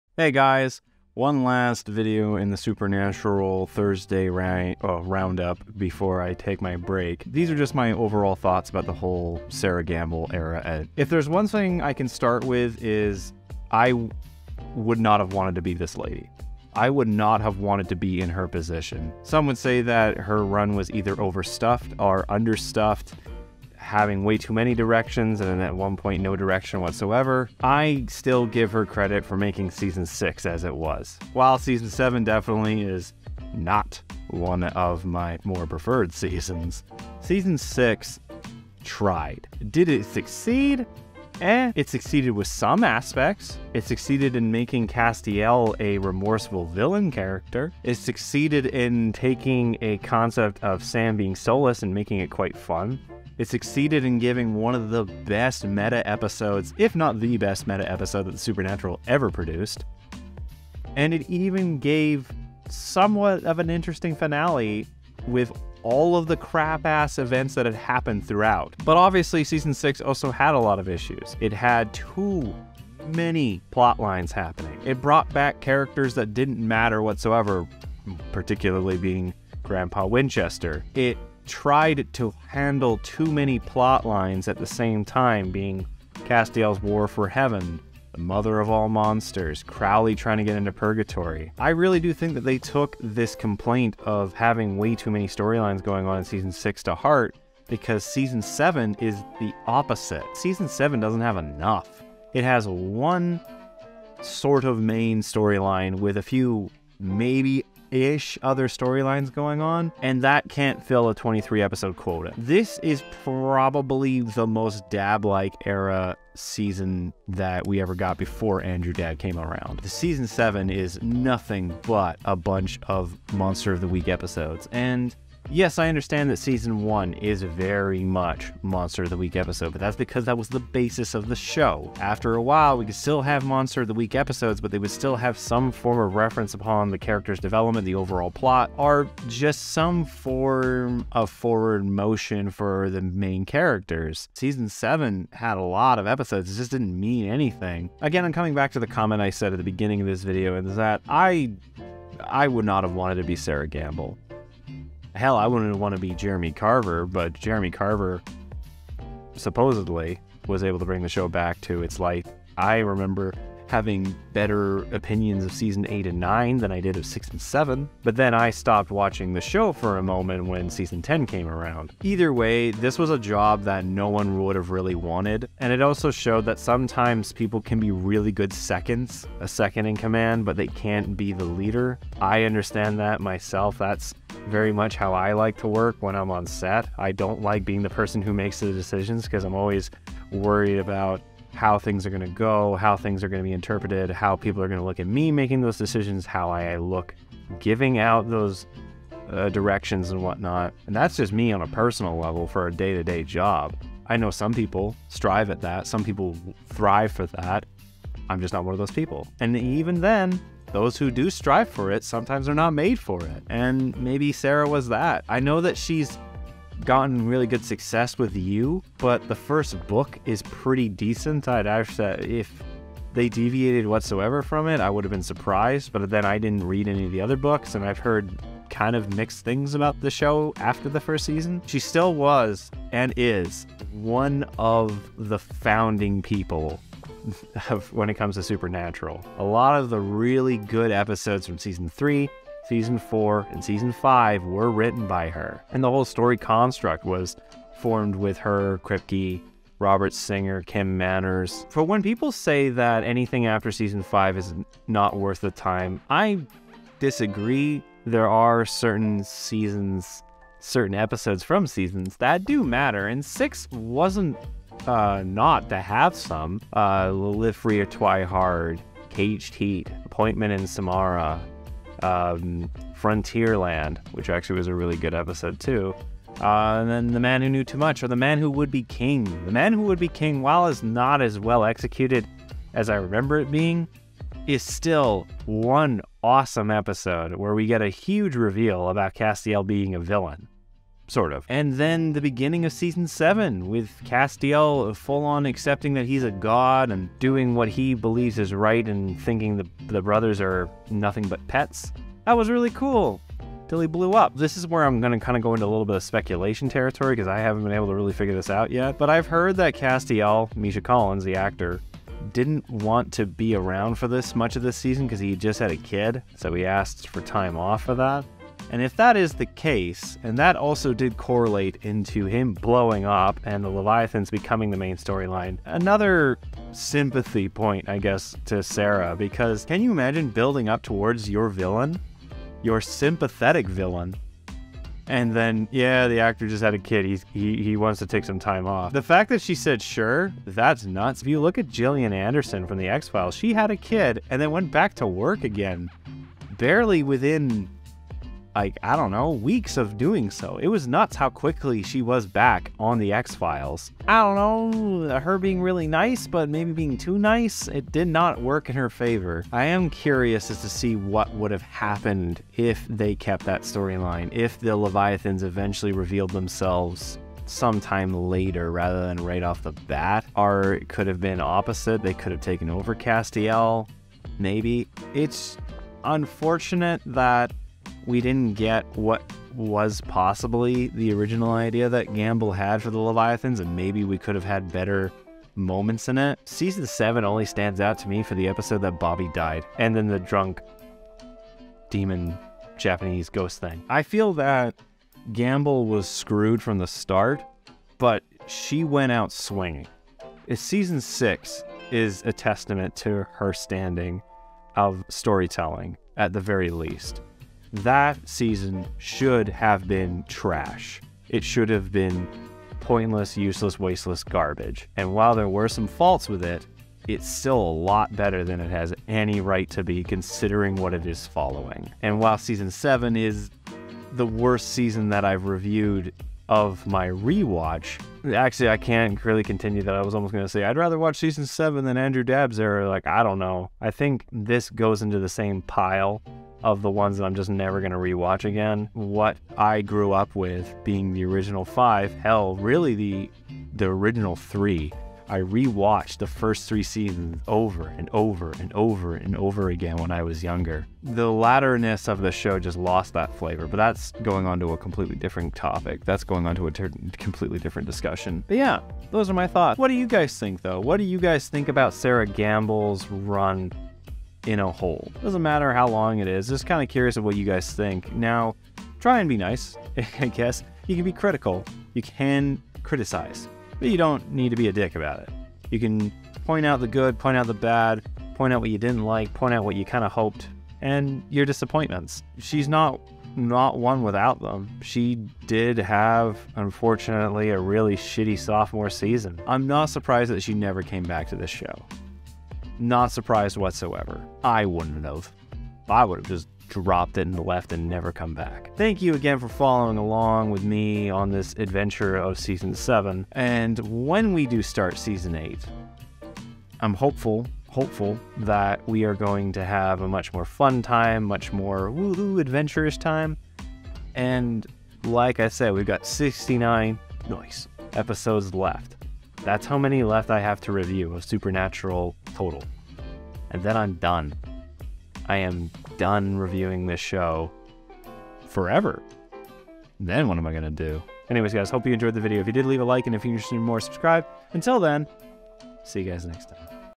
My Final Thoughts on the Sera Gamble Supernatural Era
This is it guys. My final thoughts ever on Season 6 and 7 and the complete crap sandwich of a situation Sera Gamble was given trying to follow up the true ending of Supernatural.
All and all, mistakes and achievements, she did okay considering having the job probably no one wanted.